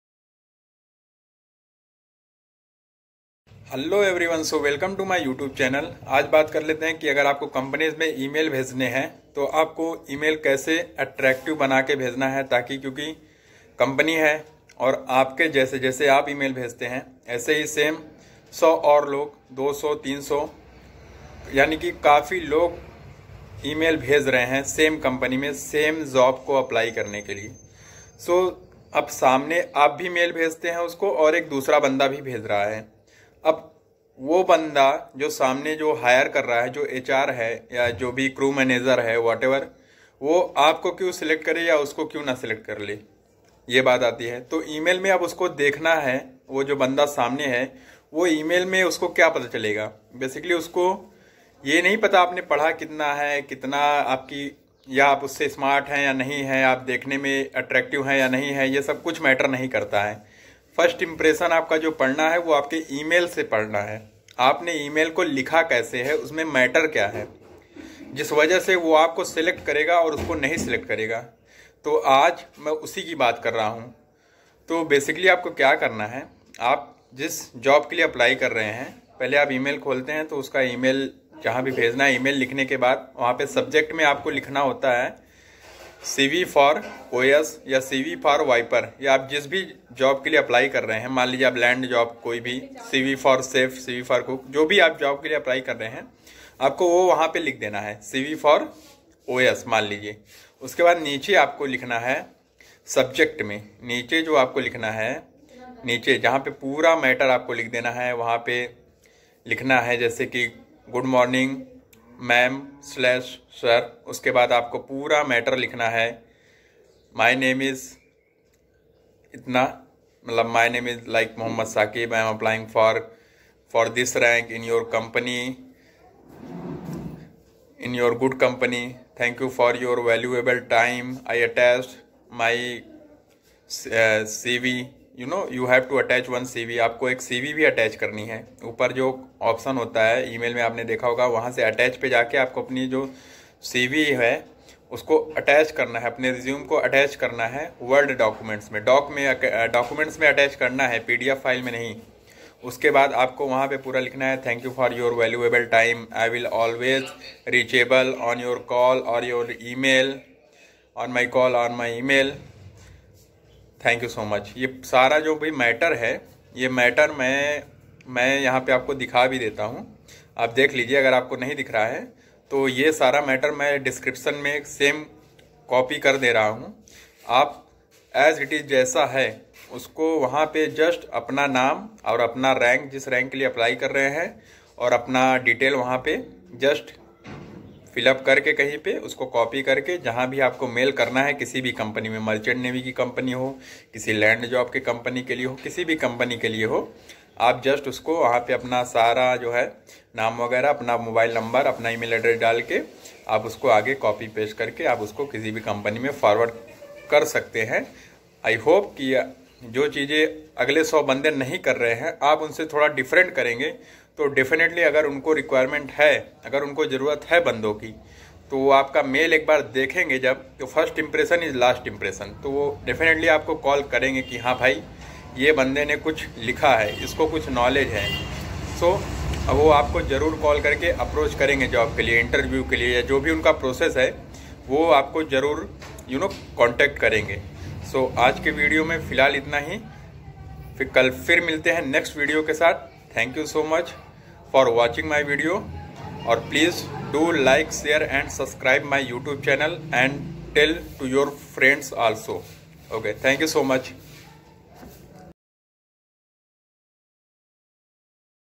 Resume ko kaise email kare||how to send email resume/cv in company for jobs in good manner.
Dear mam/sir

good morning

I am applying for "os or wiper" post in your company.

For more information I have attached my resume for your consideration.

Thank you for your valuable time. I am always reachable on my email and phone number.

Sincerely

Mohd.

+91-

please mere channel ko subscrib kare bhai and support me @shakibthesailer

Thankyou 🙂